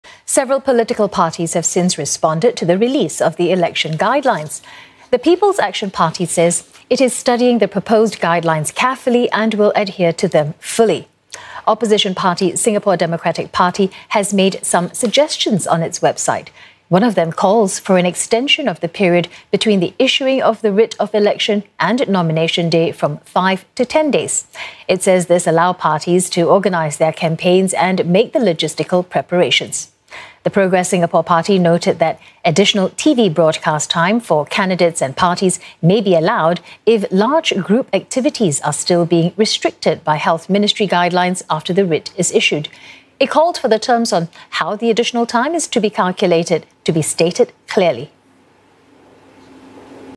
Political parties react to guidelines by Elections Department
Several political parties have responded to the release of the election guidelines.